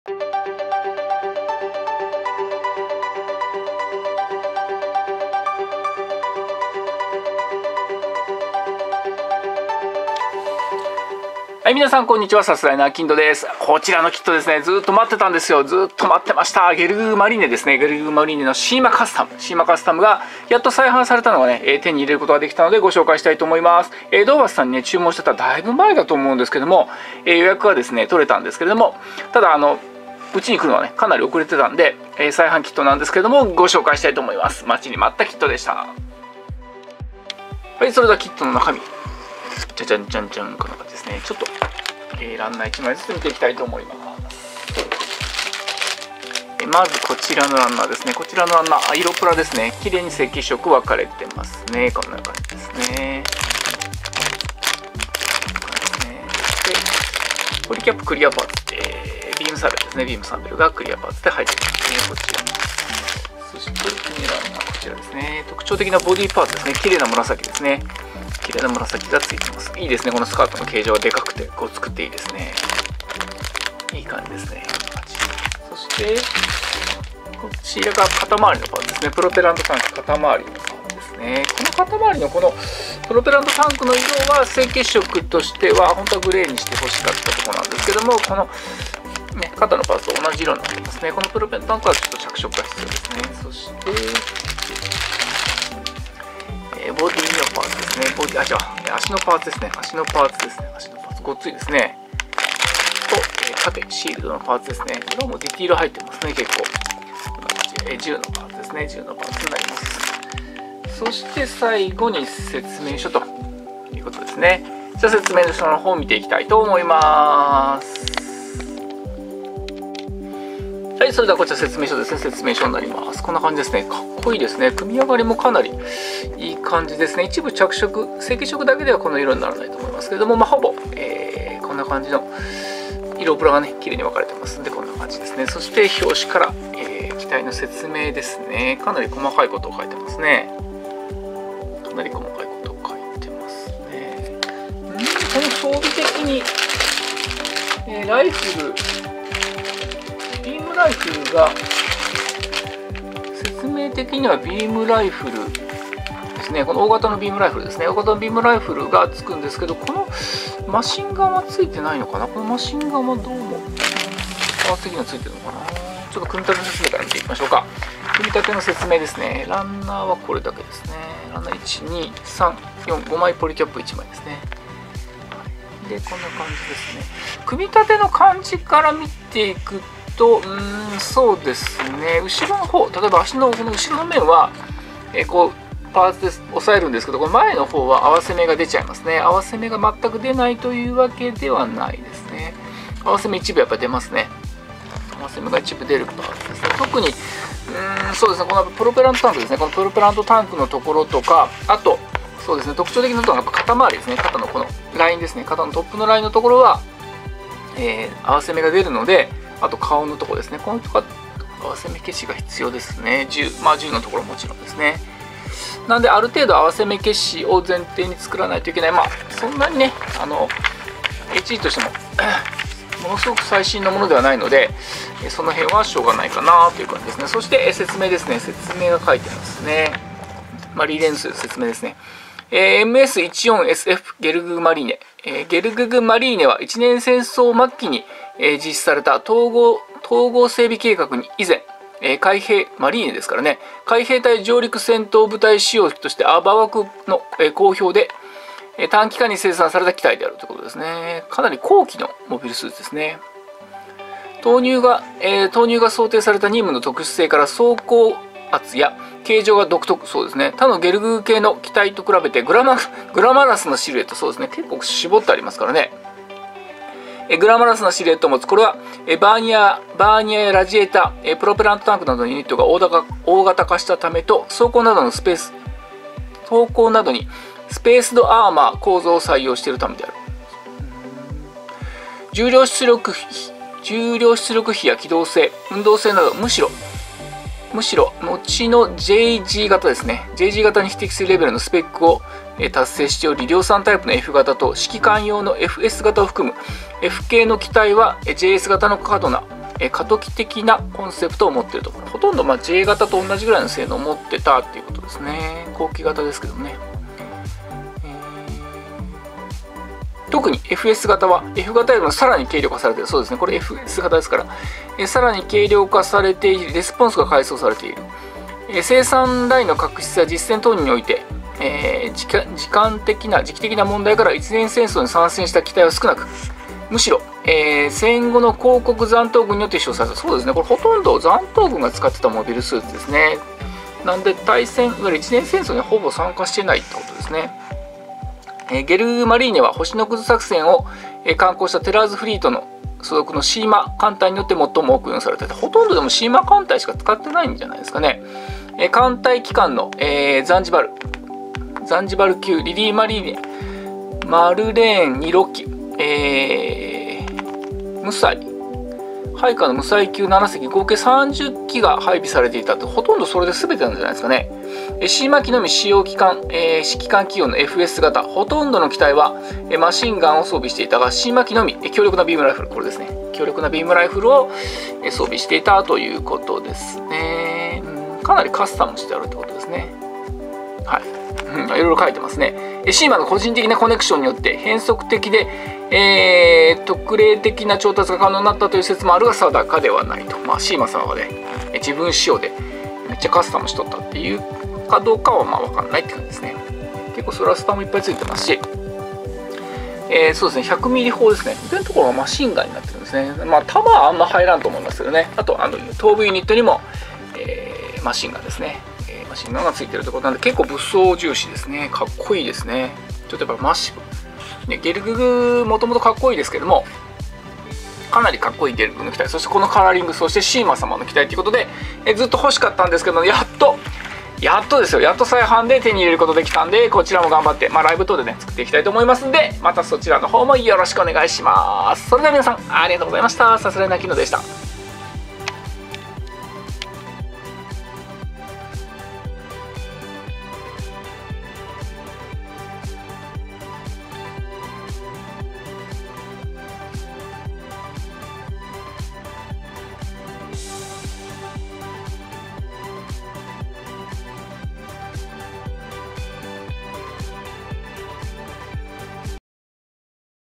ゲルググマリーネシーマカスタムＨＧガンプラ再販キットレビュー、待ってたので再販が嬉しいです😃
再販が嬉しすぎるHGのゲルググマリーネシーマカスタムです